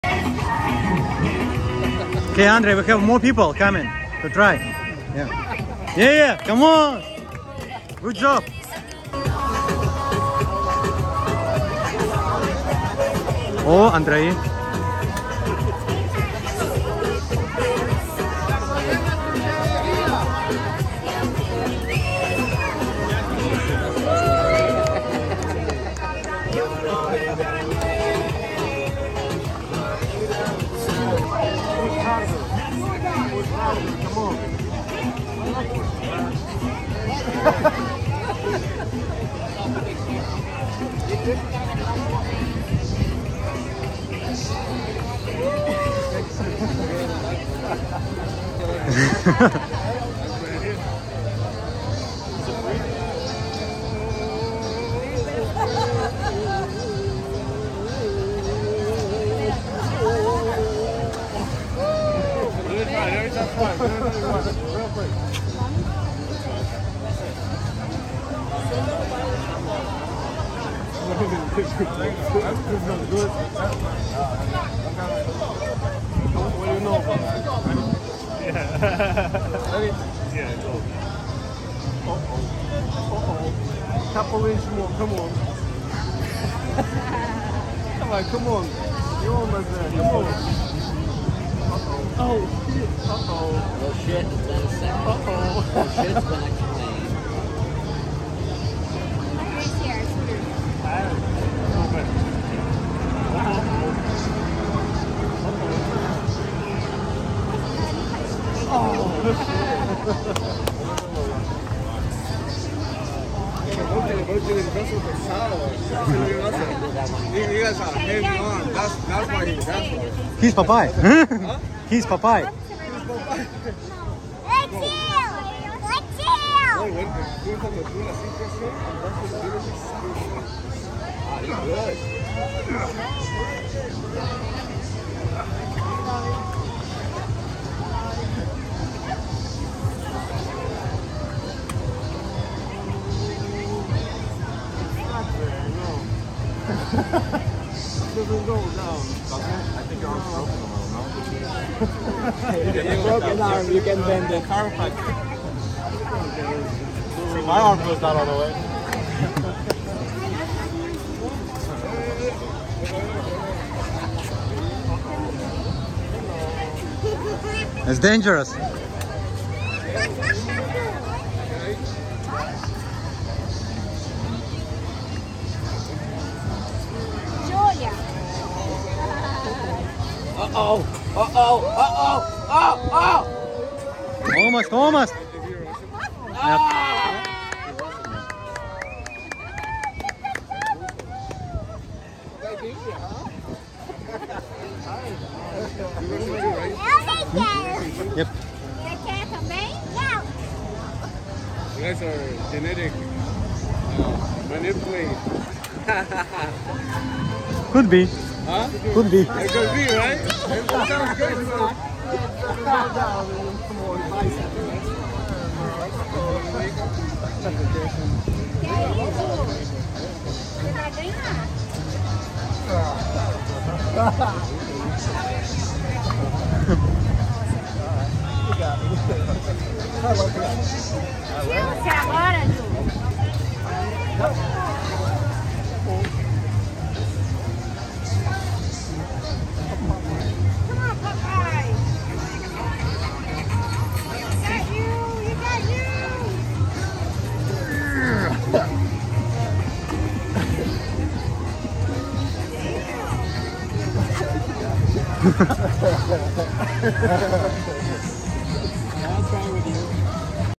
Popeye vs Random People Arm Wrestling
I challenged people at armwrestling on Hollywood Beach at the end of October 2022, I had a lot of fun with me friends Deny Ana Andrei.